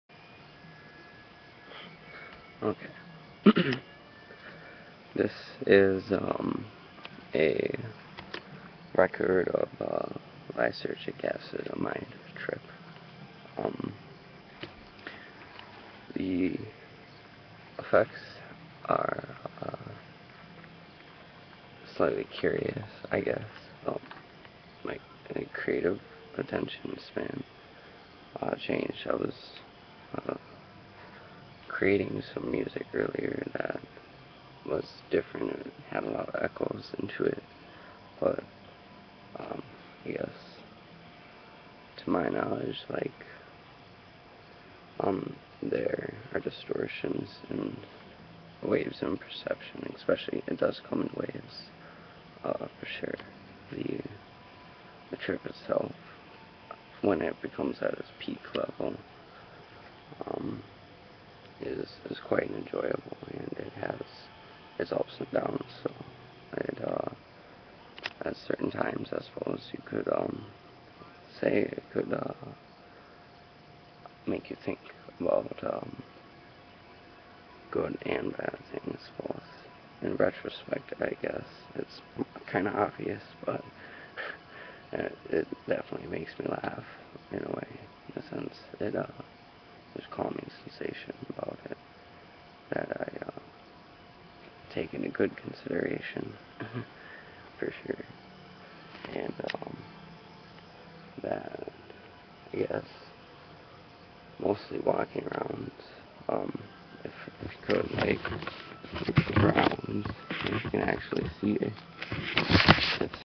Lysergic Acid Amide experiance
A short video of my experiance on Morning Glory 100 to ,150+